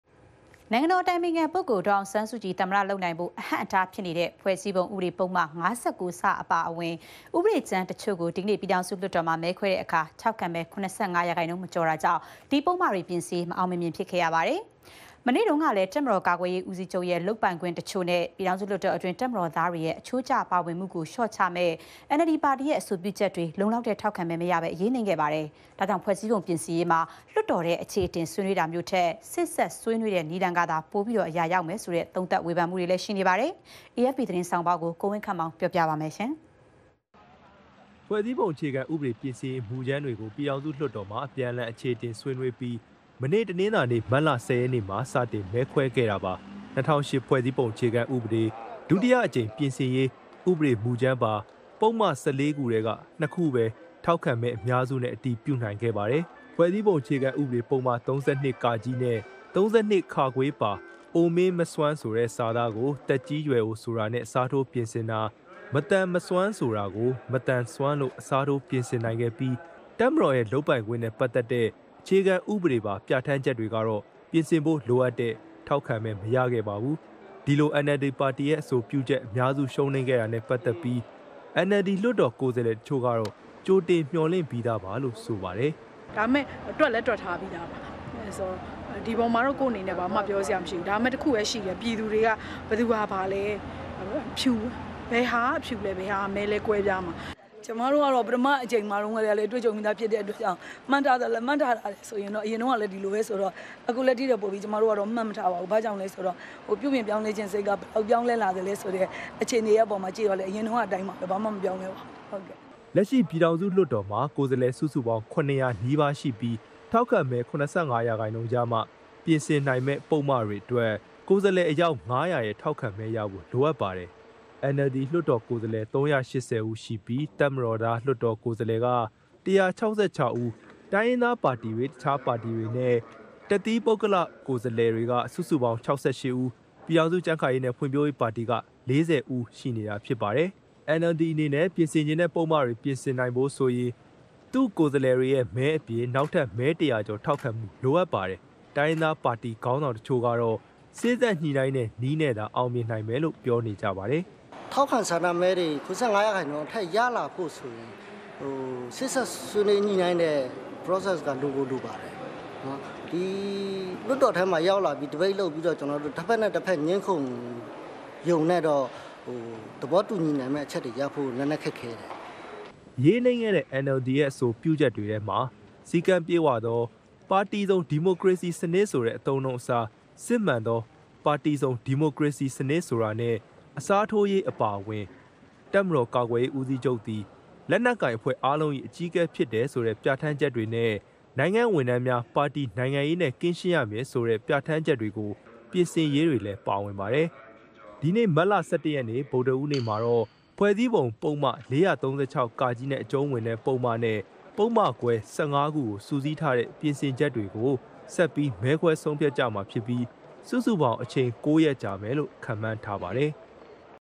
၂၀၀၈ ဖြဲ႔စည္းပုံျပင္ဆင္ေရးနဲ႔ သုံးသပ္ခ်က္တခ်ဳိ႕
ေဒၚေအာင္ဆန္းစုၾကည္ သမၼတျဖစ္ခြင့္ ဟန္႔တားထားတဲ့ ဖဲြ႔စည္းပံုဥပေဒ ပုဒ္မ ၅၉ (စ) အပါအ၀င္ ဥပေဒၾကမ္းတခ်ဳိ႕ျပင္ဆင္ေရး ျပည္ေထာင္စုလႊတ္ေတာ္မွာ မဲခြဲတာမွာ ေထာက္ခံမဲ ၇၅ % မေက်ာ္တာေၾကာင့္ မေအာင္ျမင္ခဲ့ပါဘူး။ ဖြဲ႔စည္းပံုျပင္ဆင္ေရး အလားအလာအေၾကာင္း AFP ေဆာင္းပါးတပုဒ္ကို ကို၀င္းခန္႔ေမာင္ တင္ျပထားပါတယ္။